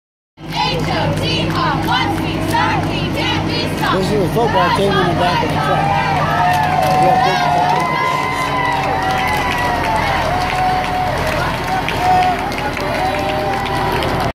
Explore Vashon Strawberry Festival 2016 Parade
Every year we have a festival on the island called Strawberry Festival. This is a short video of the grand parade... and my boy. If you are visiting Vashon Island, check out for tons of stuff to do, eat, drink and buy.